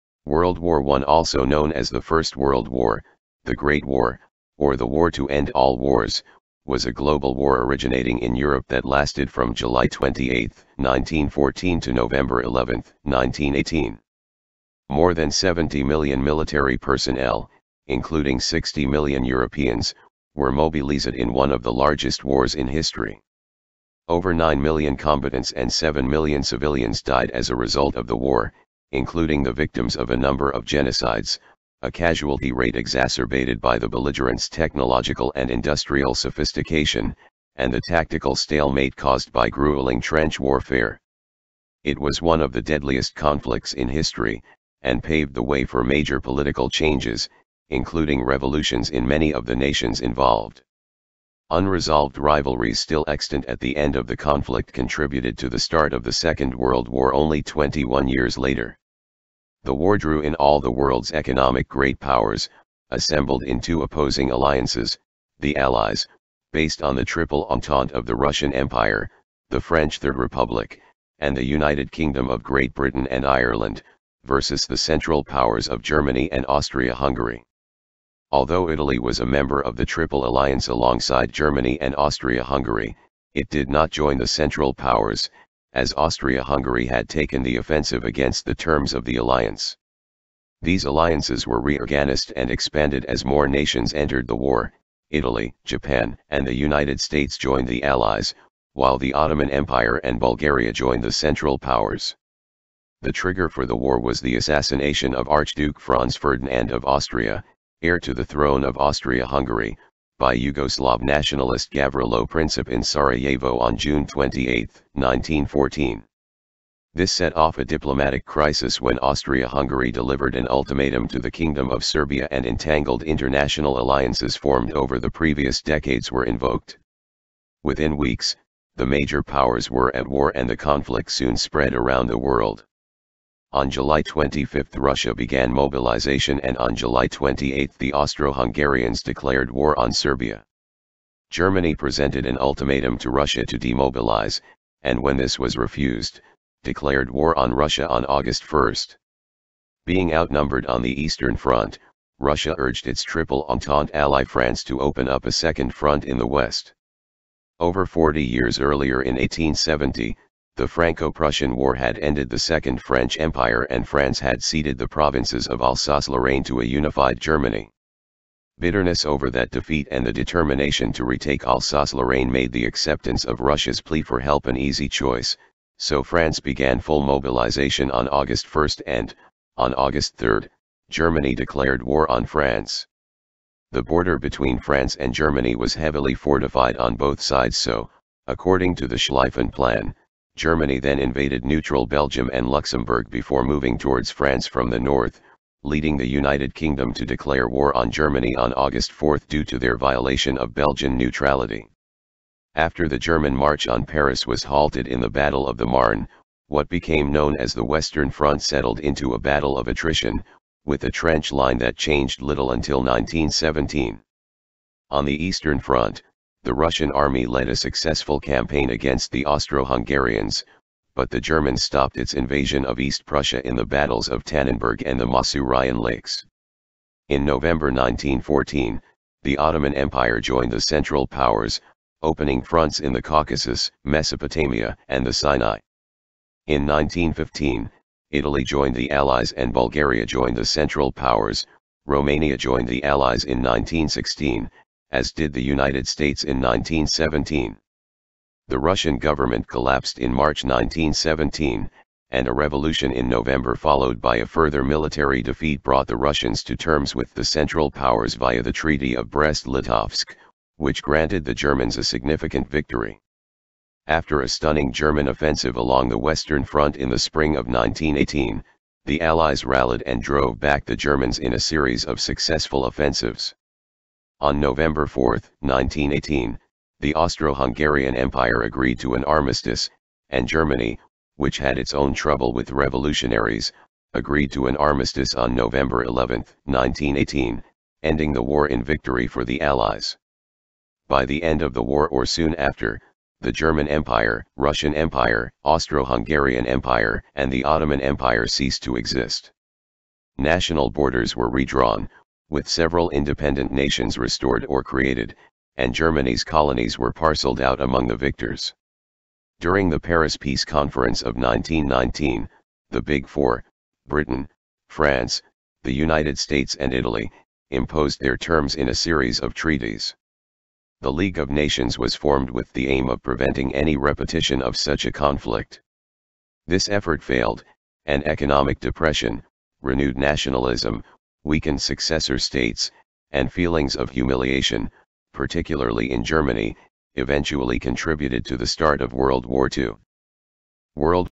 World War One
World War I also known as the First World War, the Great War, or the War to End All Wars, was a global war originating in Europe that lasted from 28 July 1914 to 11 November 1918. More than 70 million military personnel, including 60 million Europeans, were mobilised in one of the largest wars in history. Over nine million combatants and seven million civilians died as a result of the war (including the victims of a number of genocides), a casualty rate exacerbated by the belligerents' technological and industrial sophistication, and the tactical stalemate caused by gruelling trench warfare. It was one of the deadliest conflicts in history, and paved the way for major political changes, including revolutions in many of the nations involved. Unresolved rivalries still extant at the end of the conflict contributed to the start of the Second World War only twenty-one years later. The war drew in all the world's economic great powers, assembled in two opposing alliances: the Allies (based on the Triple Entente of the Russian Empire, the French Third Republic, and the United Kingdom of Great Britain and Ireland) versus the Central Powers of Germany and Austria-Hungary. Although Italy was a member of the Triple Alliance alongside Germany and Austria-Hungary, it did not join the Central Powers, as Austria-Hungary had taken the offensive against the terms of the alliance. These alliances were reorganised and expanded as more nations entered the war: Italy, Japan and the United States joined the Allies, while the Ottoman Empire and Bulgaria joined the Central Powers. The trigger for the war was the assassination of Archduke Franz Ferdinand of Austria, heir to the throne of Austria-Hungary, by Yugoslav nationalist Gavrilo Princip in Sarajevo on 28 June 1914. This set off a diplomatic crisis when Austria-Hungary delivered an ultimatum to the Kingdom of Serbia and entangled international alliances formed over the previous decades were invoked. Within weeks, the major powers were at war and the conflict soon spread around the world. On 25 July Russia began mobilisation and on 28 July the Austro-Hungarians declared war on Serbia. Germany presented an ultimatum to Russia to demobilise, and when this was refused, declared war on Russia on 1 August. Being outnumbered on the Eastern Front, Russia urged its Triple Entente ally France to open up a second front in the west. Over forty years earlier in 1870, the Franco-Prussian War had ended the Second French Empire and France had ceded the provinces of Alsace-Lorraine to a unified Germany. Bitterness over that defeat and the determination to retake Alsace-Lorraine made the acceptance of Russia's plea for help an easy choice, so France began full mobilisation on 1 August and, on 3 August, Germany declared war on France. The border between France and Germany was heavily fortified on both sides so, according to the Schlieffen Plan, Germany then invaded neutral Belgium and Luxembourg before moving towards France from the north, leading the United Kingdom to declare war on Germany on 4 August due to their violation of Belgian neutrality. After the German march on Paris was halted in the Battle of the Marne, what became known as the Western Front settled into a battle of attrition, with a trench line that changed little until 1917. On the Eastern Front, the Russian army led a successful campaign against the Austro-Hungarians, but the Germans stopped its invasion of East Prussia in the battles of Tannenberg and the Masurian Lakes. In November 1914, the Ottoman Empire joined the Central Powers, opening fronts in the Caucasus, Mesopotamia and the Sinai. In 1915, Italy joined the Allies and Bulgaria joined the Central Powers; Romania joined the Allies in 1916, as did the United States in 1917. The Russian government collapsed in March 1917, and a revolution in November followed by a further military defeat brought the Russians to terms with the Central Powers via the Treaty of Brest Litovsk, which granted the Germans a significant victory. After a stunning German offensive along the Western Front in the spring of 1918, the Allies rallied and drove back the Germans in a series of successful offensives. On 4 November 1918, the Austro-Hungarian empire agreed to an armistice, and Germany, which had its own trouble with revolutionaries, agreed to an armistice on 11 November 1918, ending the war in victory for the Allies. By the end of the war or soon after, the German Empire, Russian Empire, Austro-Hungarian Empire and the Ottoman Empire ceased to exist. National borders were redrawn, with several independent nations restored or created, and Germany's colonies were parceled out among the victors. During the Paris Peace Conference of 1919, the Big Four (Britain, France, the United States and Italy) imposed their terms in a series of treaties. The League of Nations was formed with the aim of preventing any repetition of such a conflict.